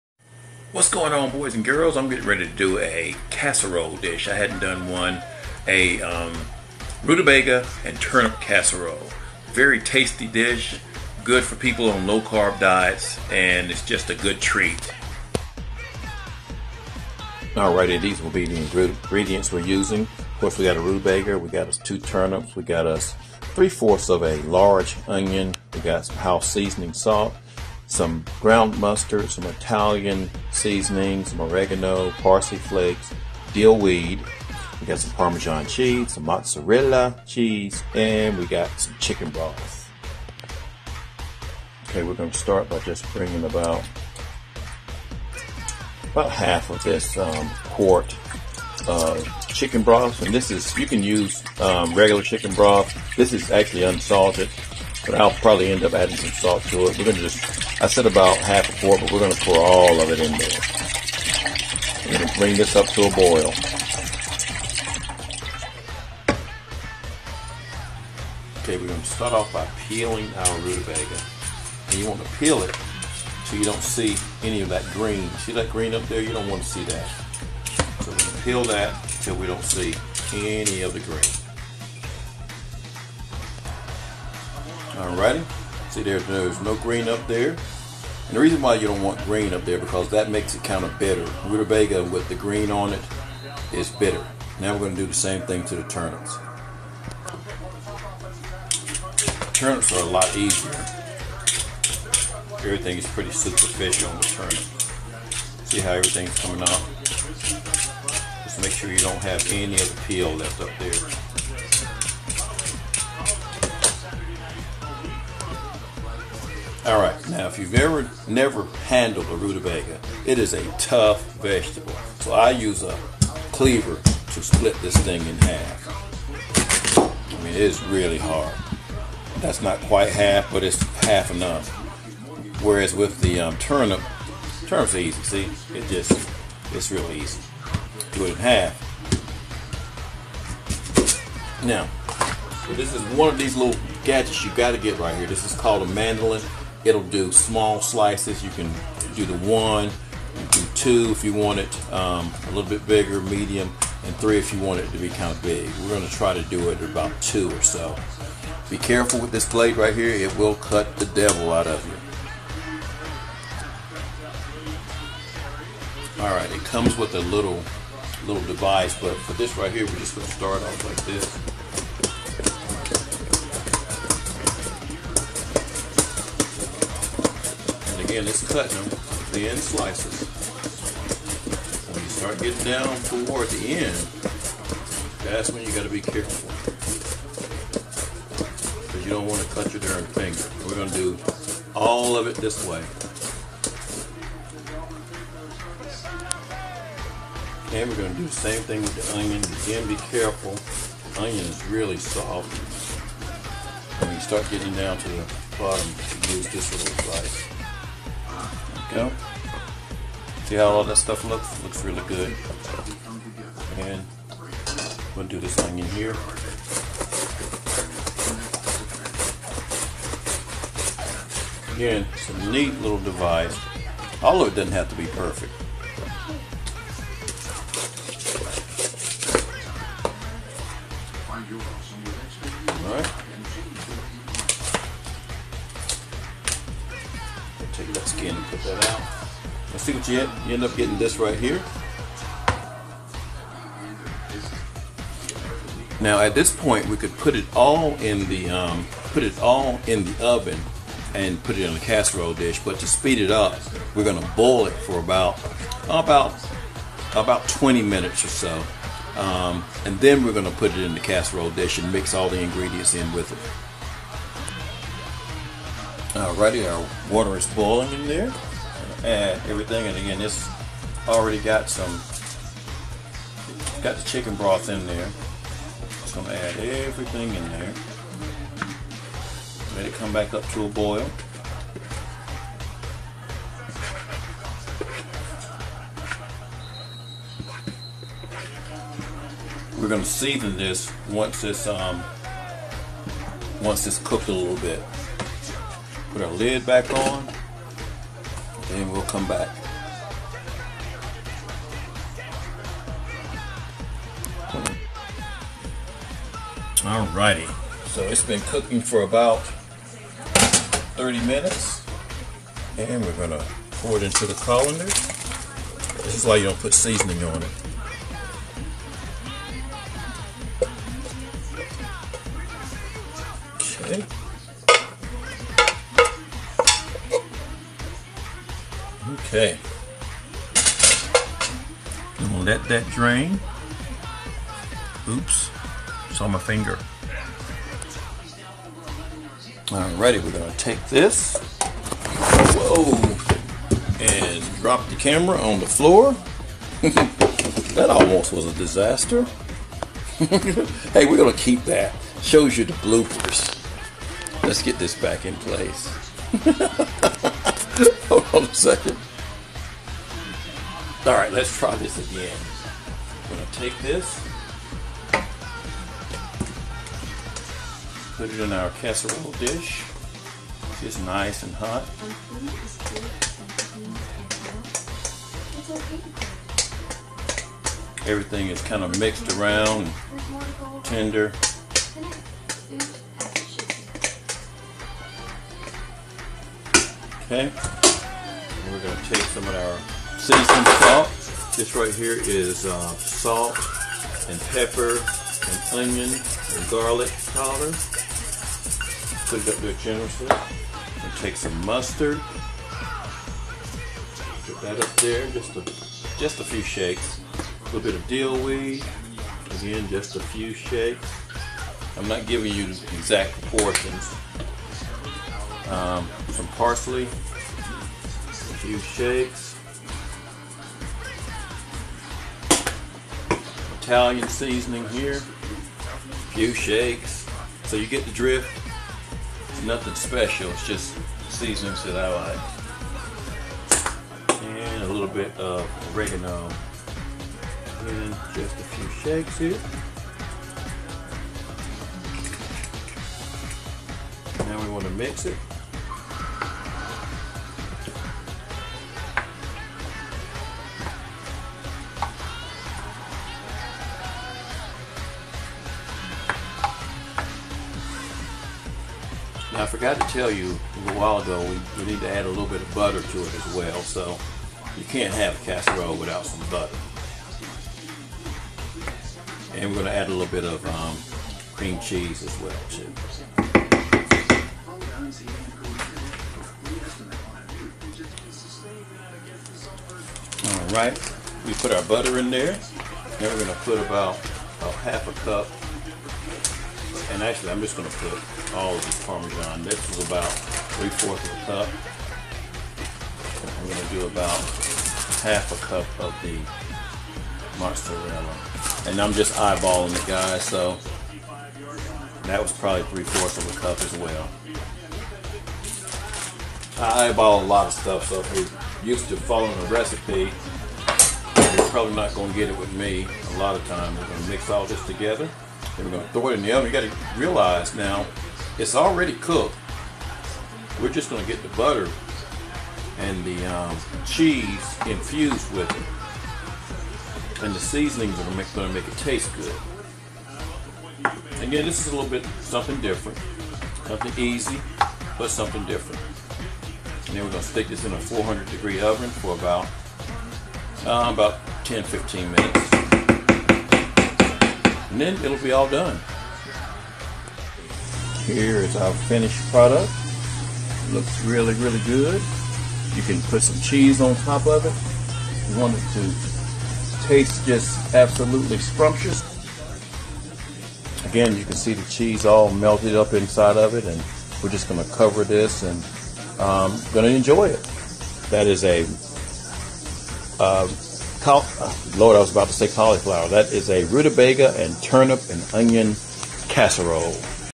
Scalloped rutabaga and turnip casserole
This is a great recipe for someone on a low-carb diet. It came out delicious and you should all try it!